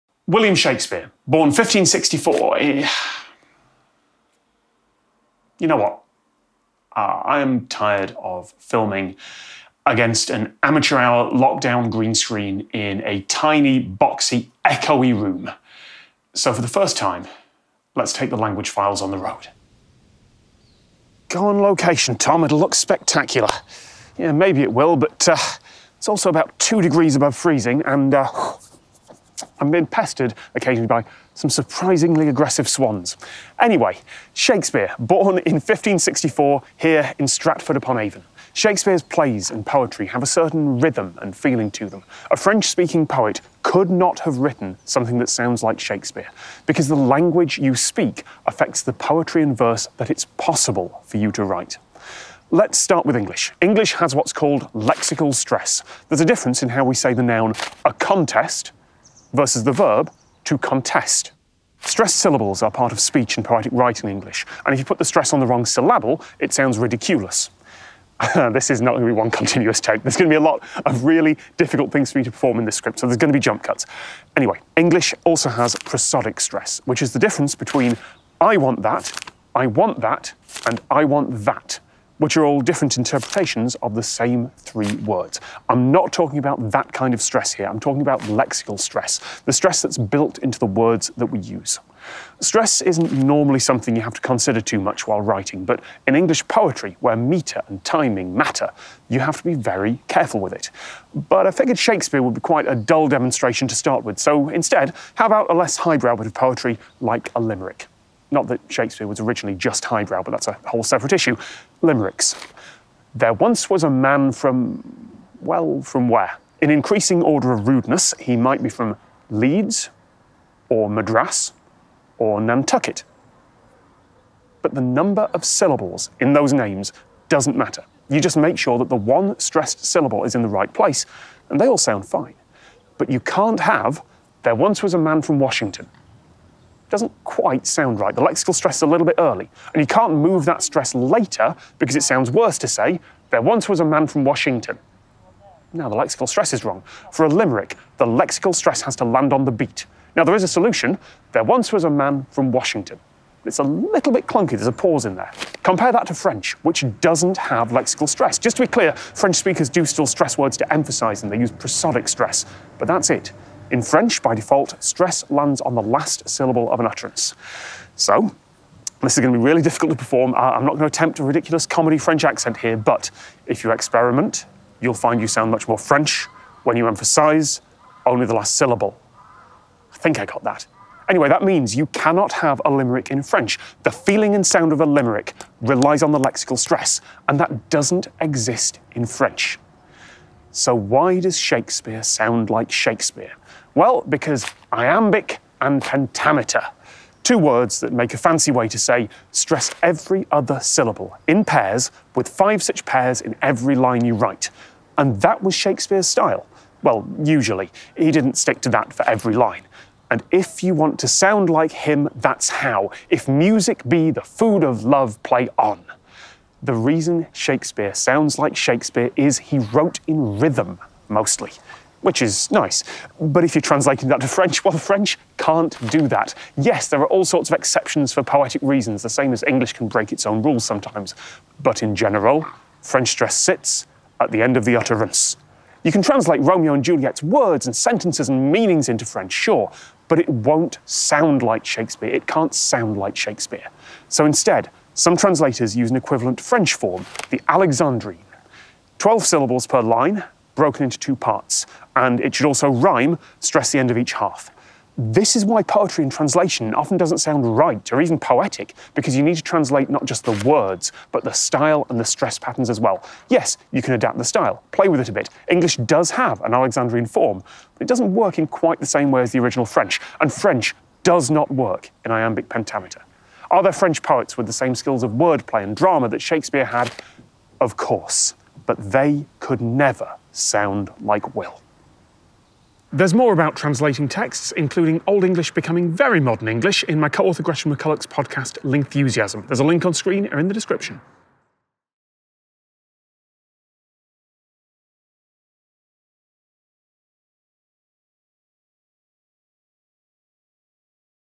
Why Shakespeare Could Never Have Been French
Shakespeare sounds a certain way. Why? And why could it only work in English? • Written with Gretchen McCulloch of Lingthusiasm! Her podcast has an episode about how translators approach texts

Gretchen's book BECAUSE INTERNET, all about the evolution of internet language, is available: